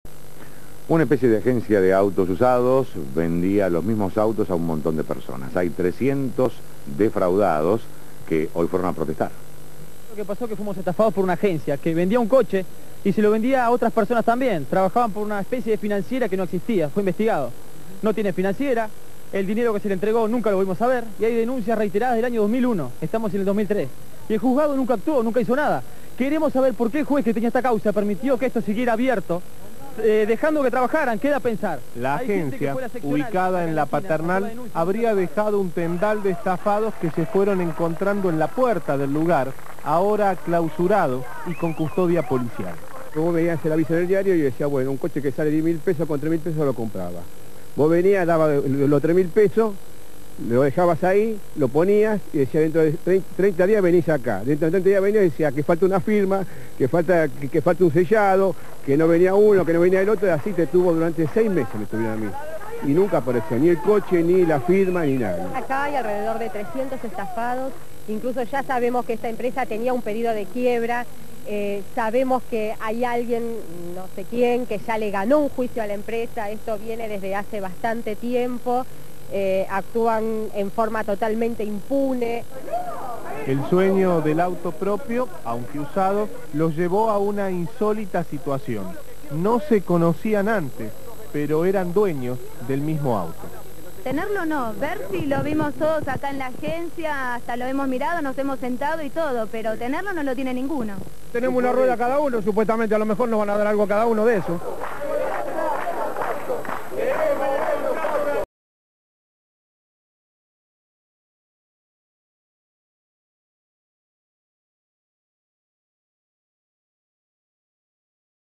Estafa - agencia vendia el mismo a auto a decenas de personas 2003 V-05017 - DiFilm
Fecha: 30/7/03 30 de julio del 2003
Código: V-05017